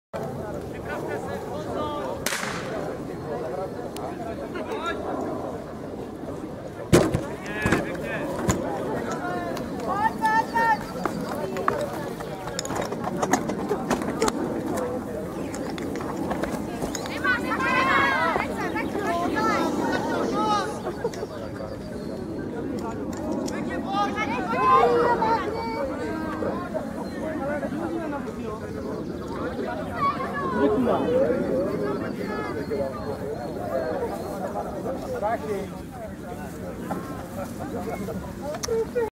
SDH Malé Přílepy Modré - 25.5.2013 - Okrsková soutěž PS v Chyňavě
čas 4. štafety 33,75 s